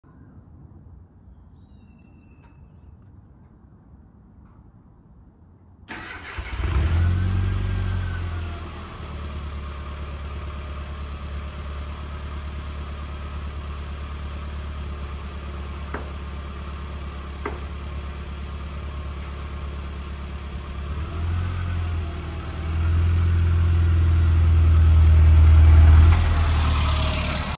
1995 Rover Mini on C&B - Cold start
Cold start video for auction purposes.